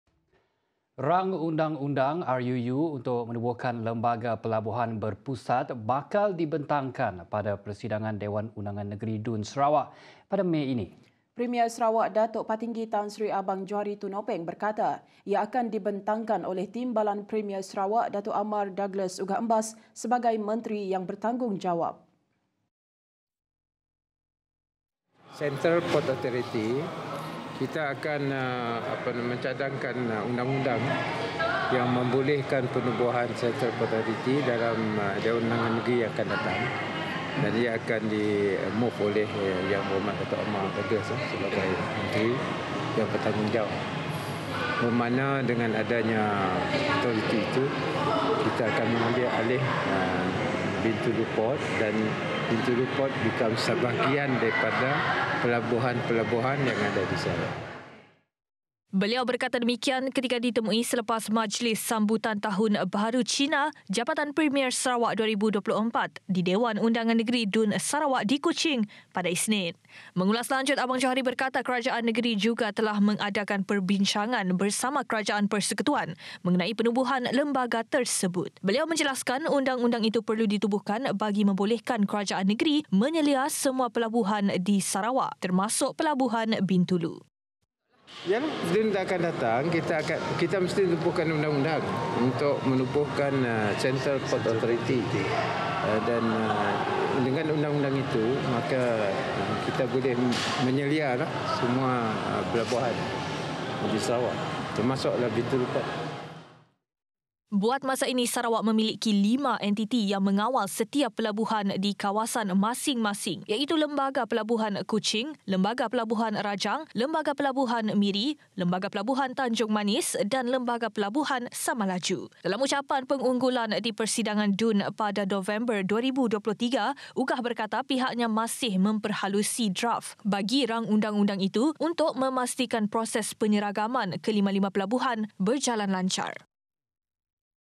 RUU penubuhan Lembaga Pelabuhan Berpusat dibentang di DUN Mei ini
Rang undang-undang (RUU) untuk menubuhkan Lembaga Pelabuhan Berpusat bakal dibentangkan pada persidangan Dewan Undangan Negeri (DUN) Sarawak pada Mei ini.

Premier Sarawak Datuk Patinggi Tan Sri Abang Johari Tun Openg berkata, ia akan dibentangkan oleh Timbalan Premier Sarawak, Datuk Amar Douglas Uggah sebagai menteri yang bertanggungjawab bagi menyelesaikan perkara itu.